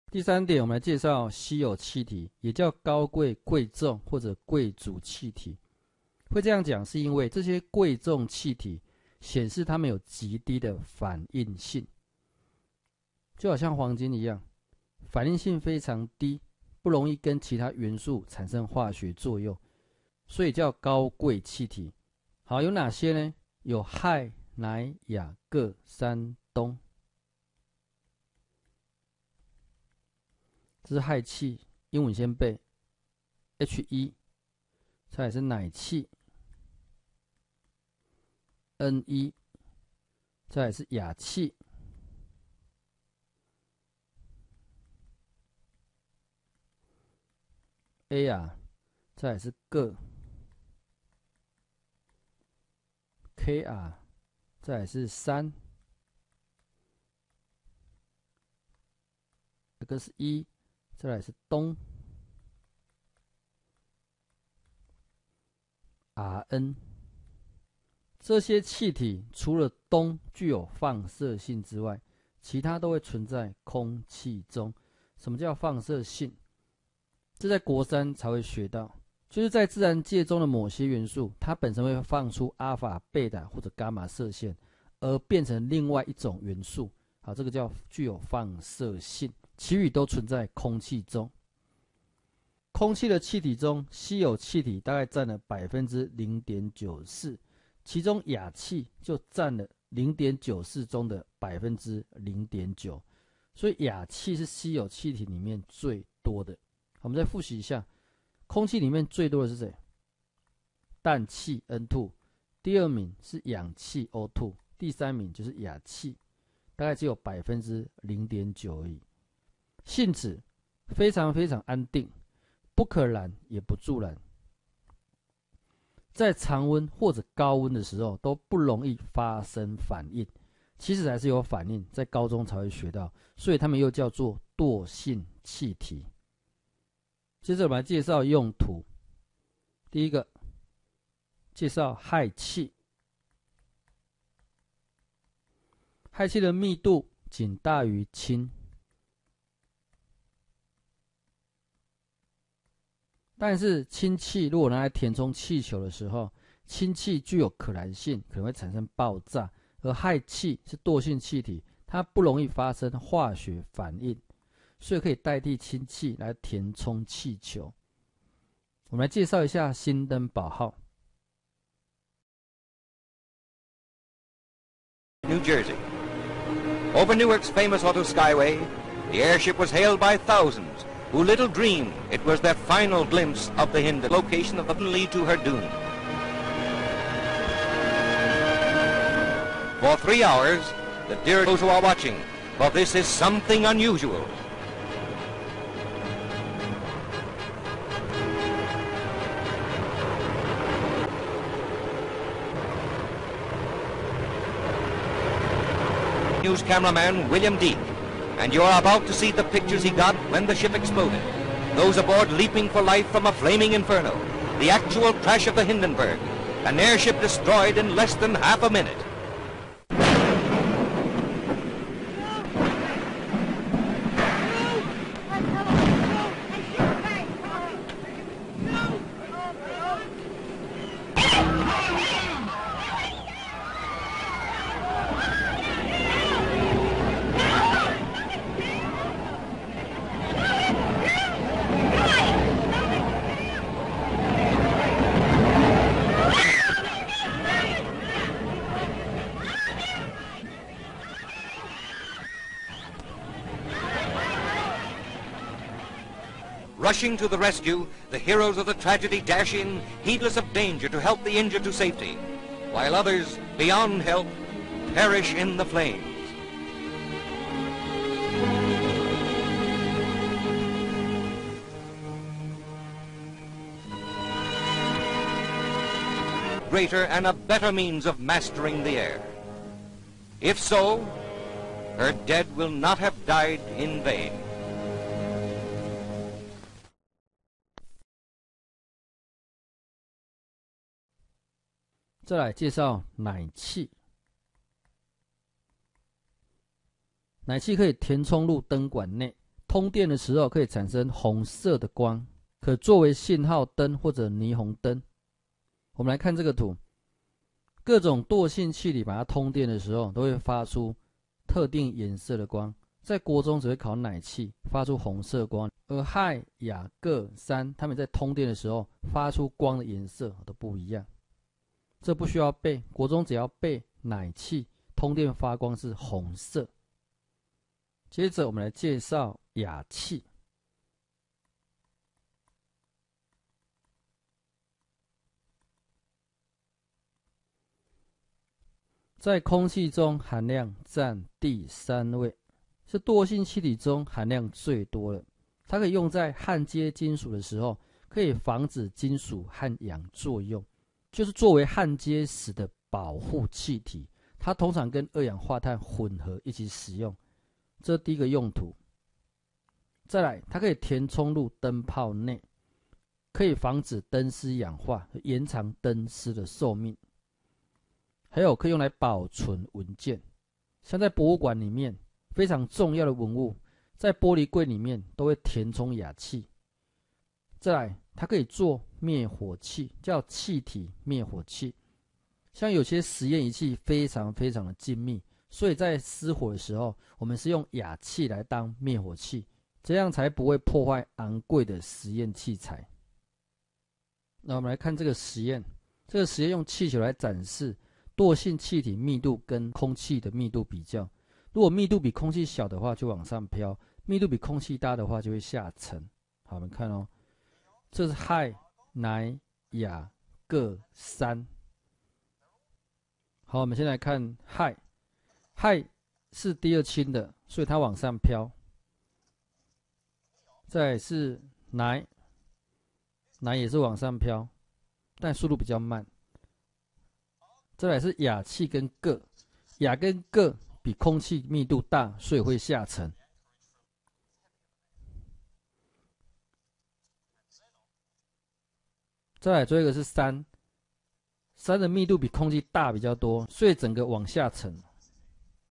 國二理化_惰性氣體_高貴氣體_稀有氣體【國中理化】
「貴重氣體」(noble gases)顯示它們極低的反應性。此名稱的含意與黃金等的「貴金屬」類似，有財富、高貴及低反應性的意思。
(1)存在：包括氦（He）、氖（Ne）、氬（Ar）、氪（Kr）、氙（Xe）、氡（Rn），除了氡具放射性外，其餘均存在於空氣中。

完整免費教學影音課程+教學講義下載請至莫斯利國中自然科學官網